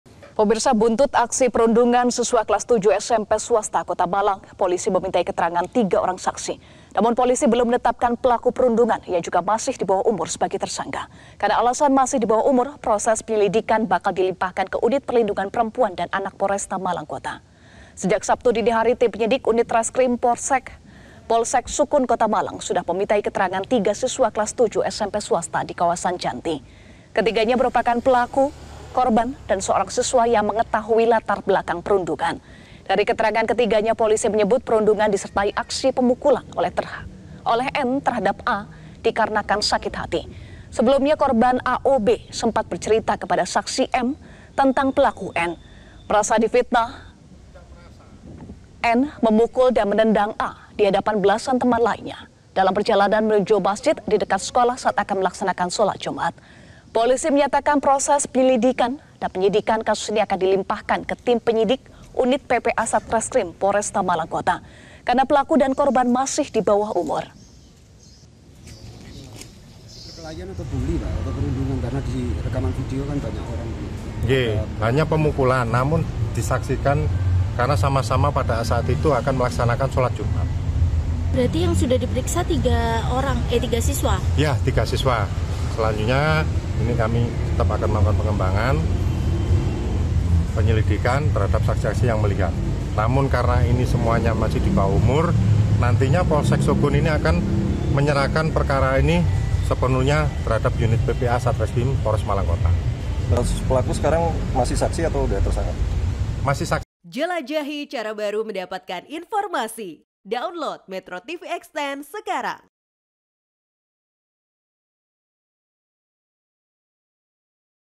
Polisi Periksa Pelaku Aksi Siswa SMP di Malang
MetroTV, Polisi Periksa Pelaku korban dan juga saksi akibat Aksi Siswa SMP di Malang dan polisi belum tetapkan pelaku sebagai tersangka